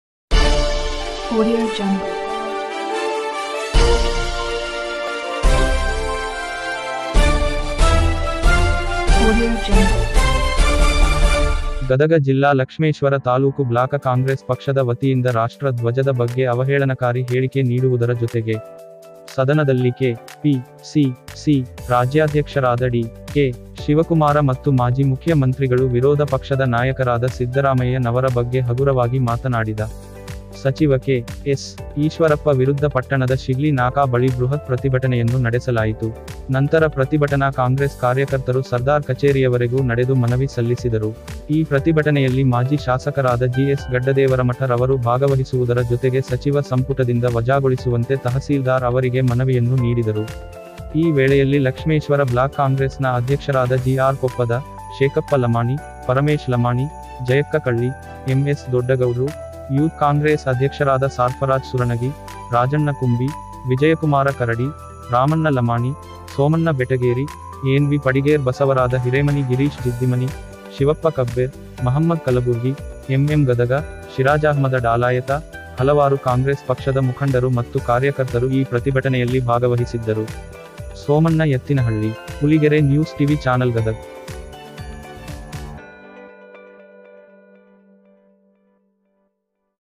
ಸಚಿವ ಕೆ ಎಸ್ ಈಶ್ವರಪ್ಪ ರಾಷ್ಟ್ರಧ್ವಜಕ್ಕೆ ಅವಮಾನ ಖಂಡಿಸಿ ಕಾಂಗ್ರೆಸ್ ಪಕ್ಷದ ವತಿಯಿಂದ ಪ್ರತಿಭಟನೆ
ಪುಲಿಗೆರೆ ನ್ಯೂ ಟಿವಿ ಚಾನಲ್ ಅನ್ನು ಲೈಕ್ ಮಾಡಿ ಸಬ್ಸ್ಕ್ರೈಬ್ ಮಾಡಿ ಶೇರ್ ಮಾಡಿ ಕಮೆಂಟ್ ಮಾಡಿ